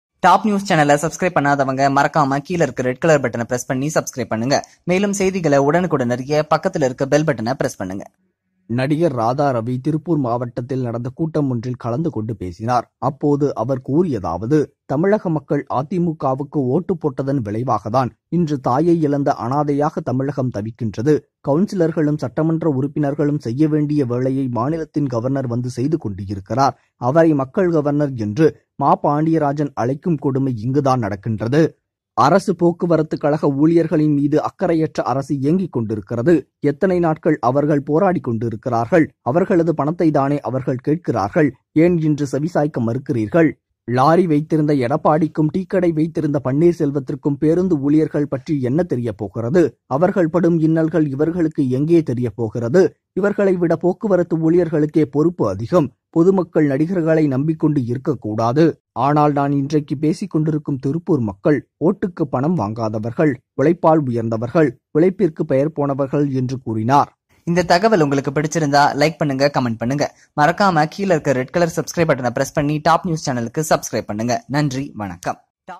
அடேங்கப்பா ! ஒபிஎஸ், இபிஎஸ், ரஜினி அனைவரையும் கிழித்த ராதாரவி ! Radha Ravi, OPS, EPS, Rajni | News
This video is about the latest speech by Actor Radha Ravi about Rajnikanth political entry and Admk party !